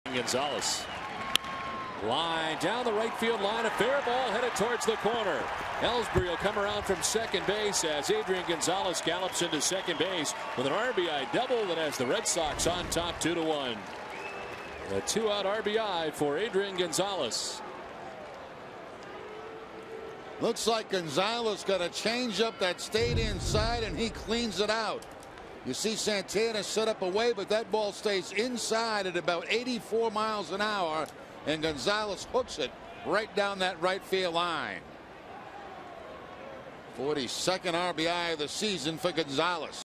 2011/05/24 Adrian's RBI double
BOS@CLE: Adrian lines an RBI double to right field